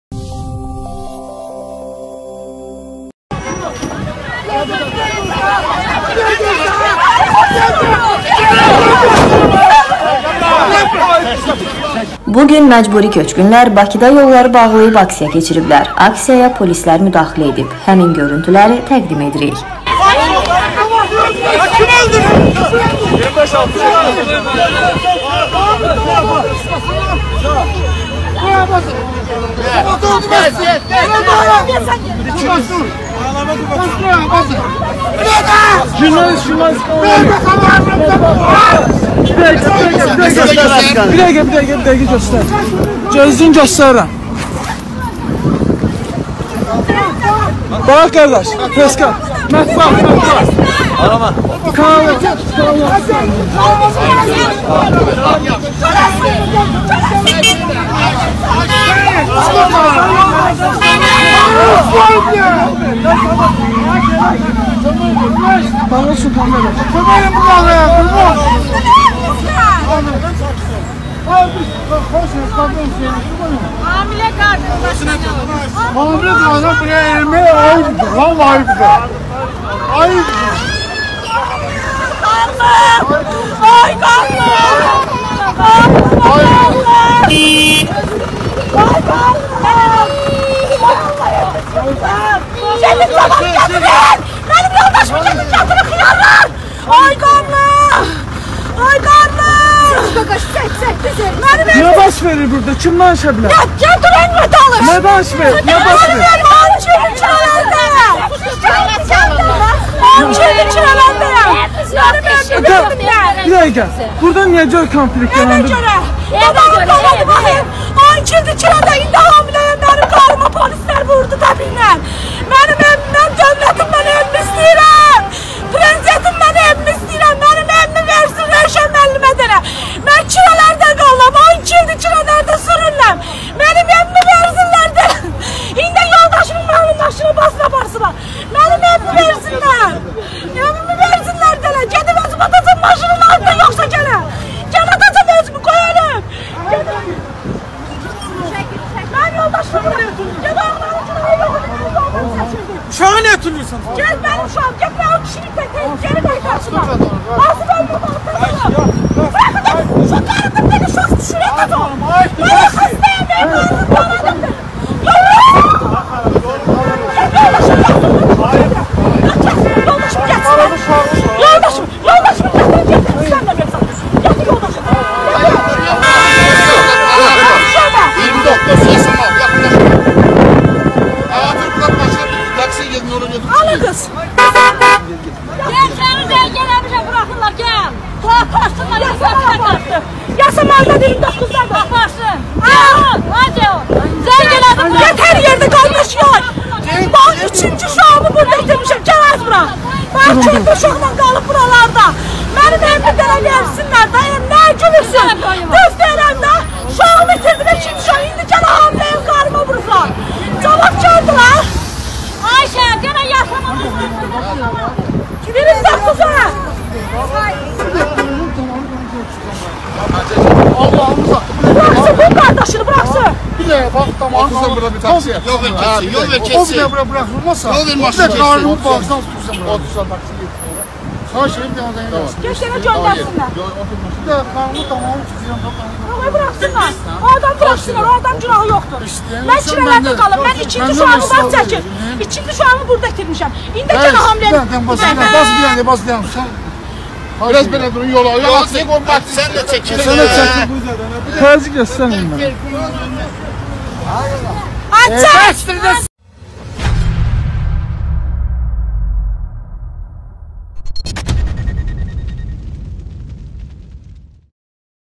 BAKIDA məcburi köçkünlər polislə üz-üzə:"Bu dəqiqə qarnımı cırıb, bağırsaqlarımı tökəcəm çolə"
Bakıda Məcburi köçkünlərlə polis arasında insident yaşanıb. Həmin görüntüləri təqdim edirik!